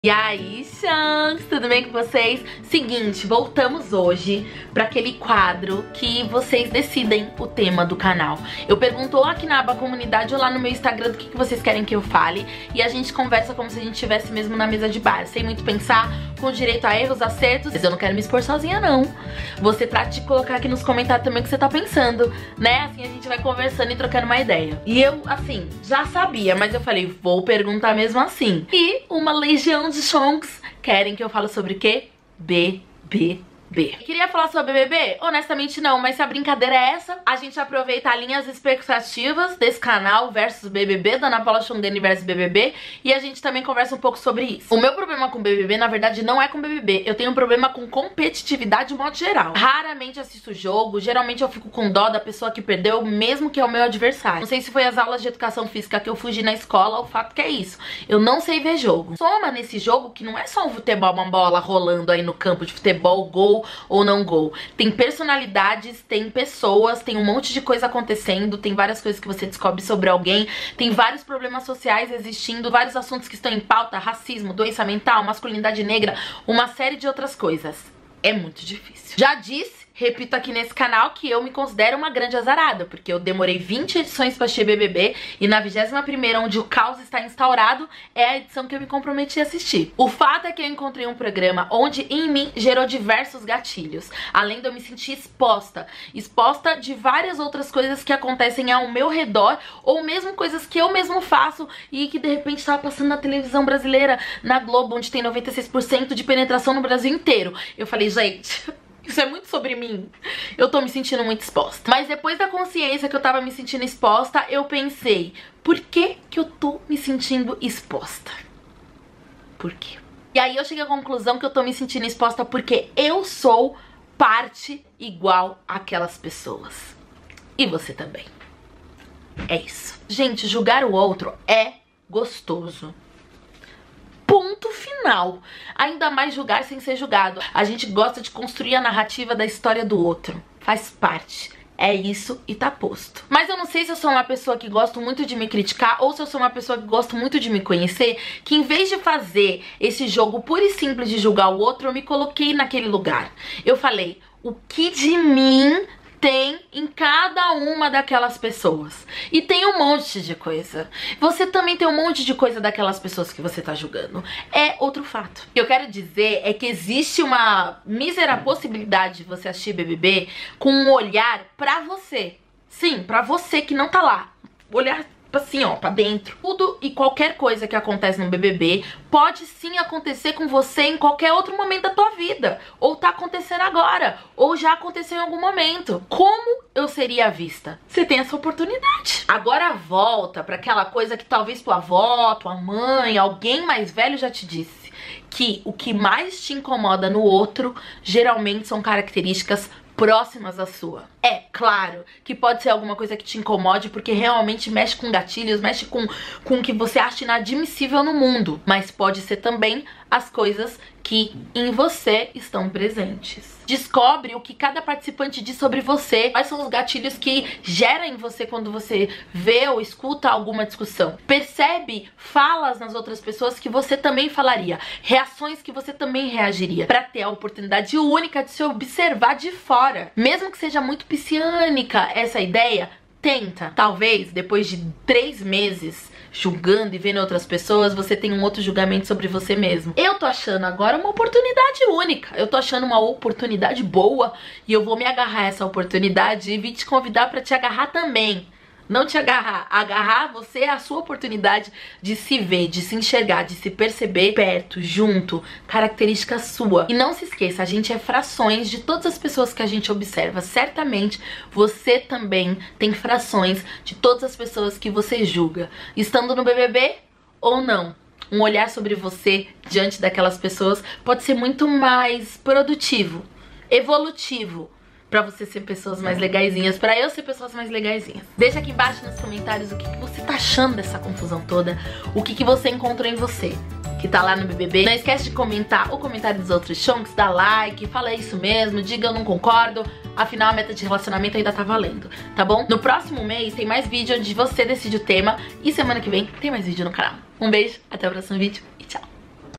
BBB21 - Caos, julgamentos... e vc nisso tudo | Ana Paula Xongani
Xongs, estamos no quadro onde vcs escolhem o tema... e é claro que você escolheram BBB21... entao vamos la! da o play!!
Nao esquece de comentar o comentário dos outros xongs... ok?!

Não parem por aqui, conversas gostosinhas também nas outras redes.

Meu insta: @anapaulaxongani
Meu face: Ana Paula Xongani